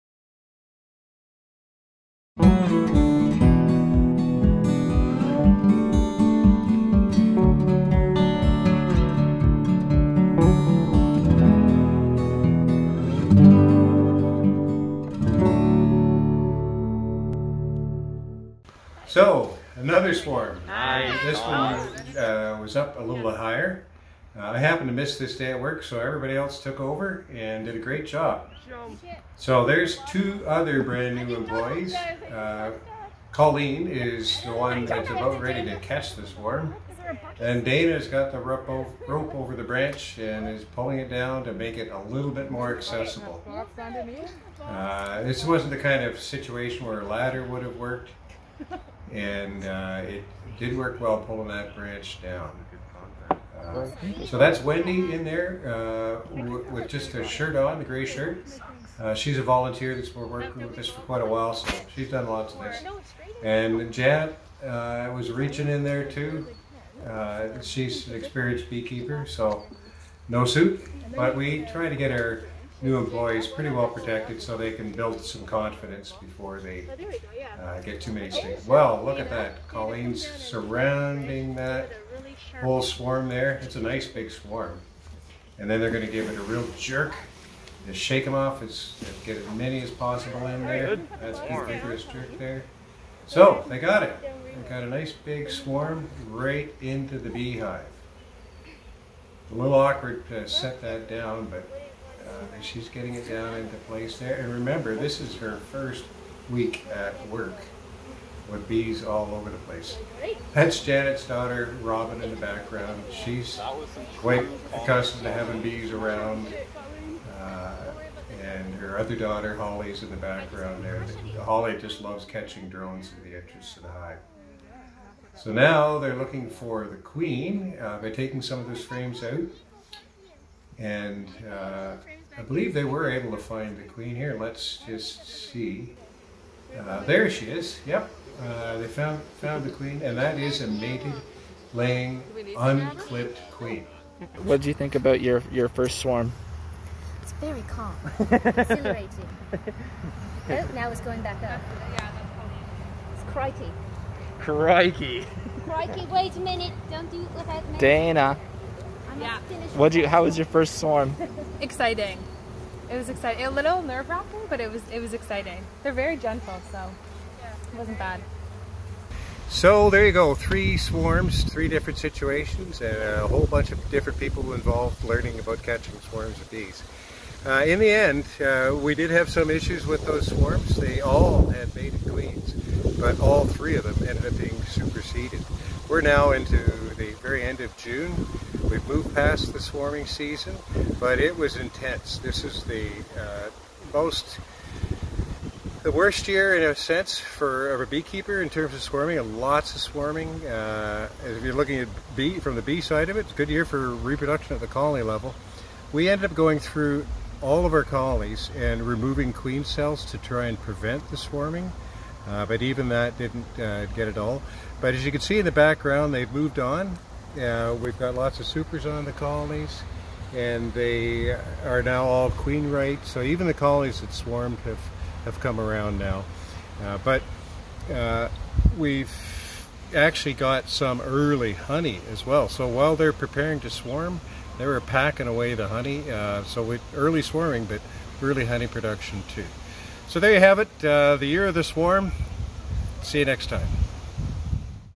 Catching a Swarm Part 3
Paul Kelly, research and apiary manager, demonstrates how to catch swarms.

For more videos, check out the University of Guelph's HBRC YouTube channel at this link:  @University of Guelph Honey Bee Research Centre